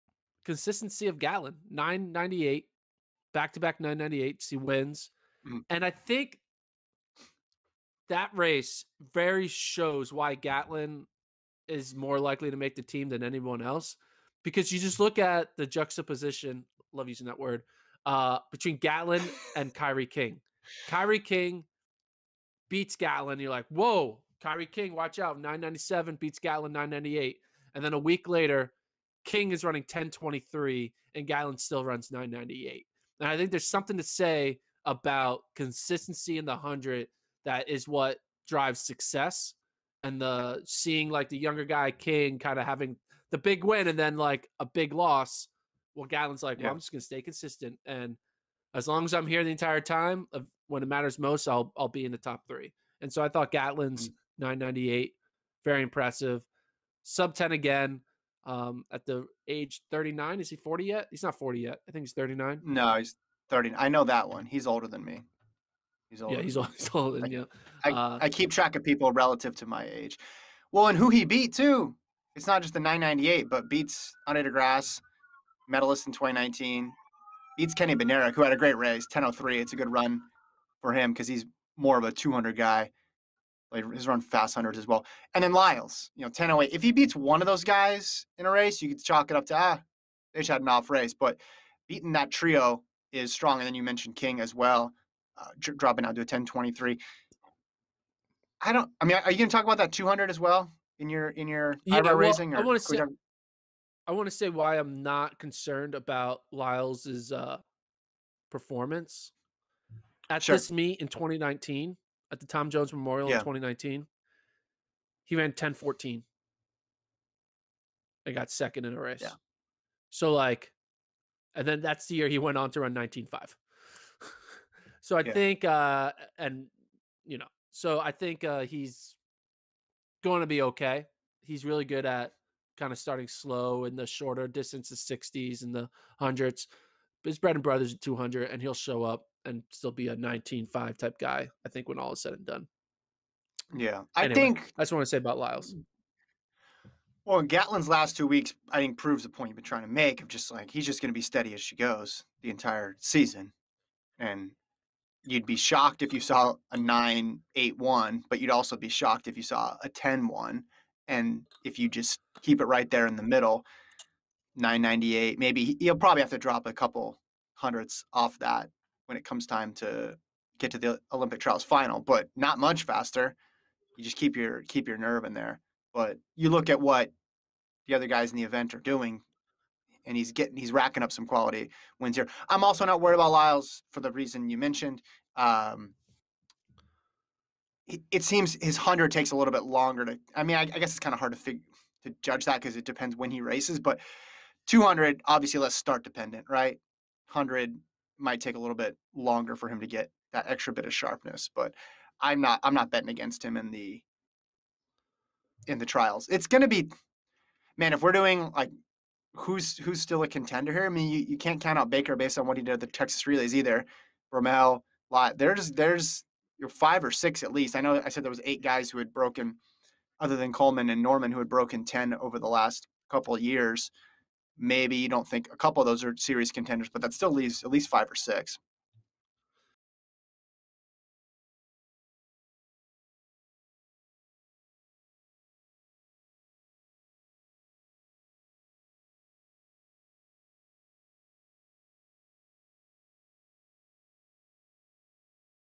REACTION: Justin Gatlin Beats Lyles, de Grasse, Bednarek In 100m Race
Discussing what Justin Gatlin's win at the 2021 Tom Ford Memorial Invitational means for the men's 100m.